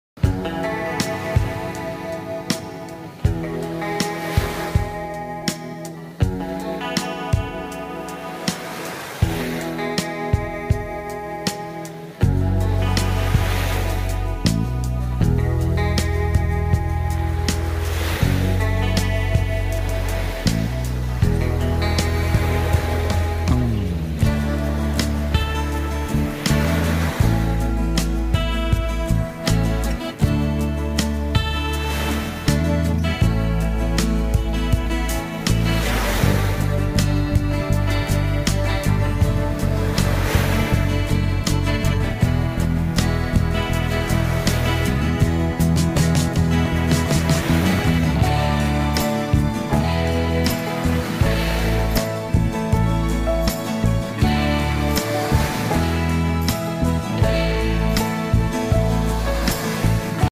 Рассвет у моря. Бургас, Болгария.
Чаще всего у меня получается застать закат для съемки, потому что рано вставать я не очень люблю. Но в тот раз я удачно заглянула к морю и получила массу удовольствия от увиденного. Наслаждайтесь и вы ☀️🌅🌞😊